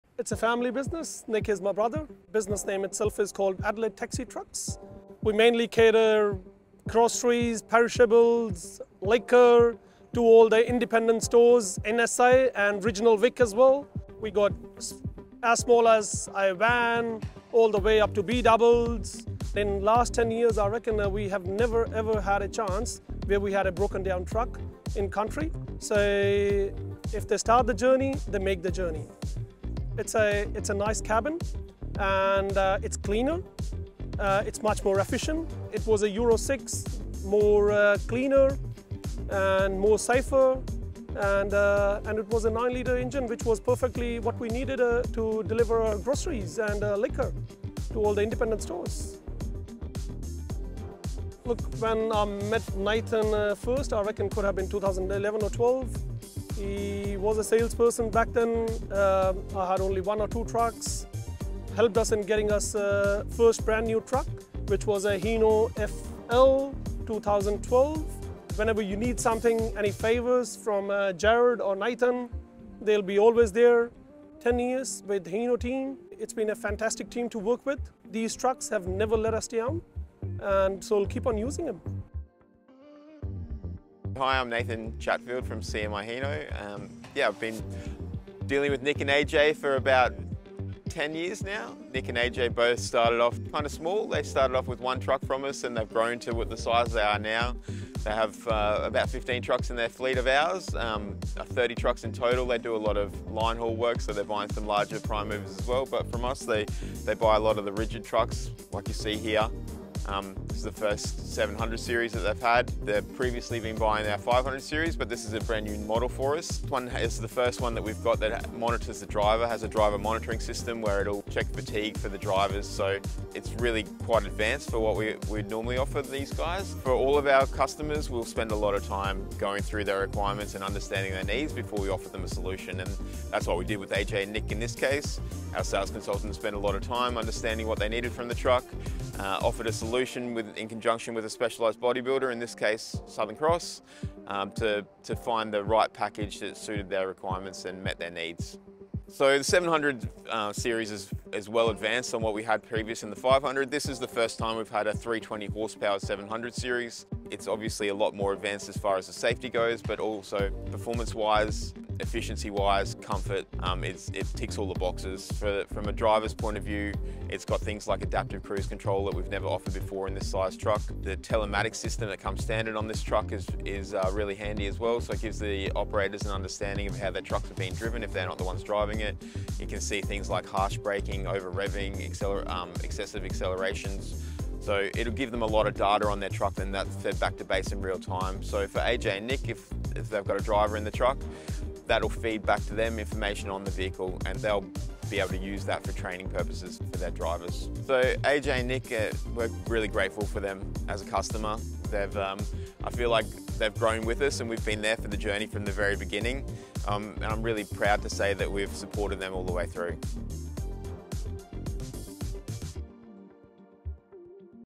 A New Hino 700 Series for Adelaide Taxi Trucks
"10 years with the CMI Hino team, it's been a fantastic team to work with. These trucks have never let us down, so we'll keen using them"

Hear from our long-time customer, AJ of Adelaide Taxi Trucks and our General Manager, Nathan, as they take delivery of their latest addition to their fleet - the Hino 700 Series! 👌

CMI Hino Adelaide has all of your needs covered.

REGENCY PARK
569 South Road
Regency Park SA 5010

LONSDALE
11-15 Lindsay Road
Lonsdale SA 5160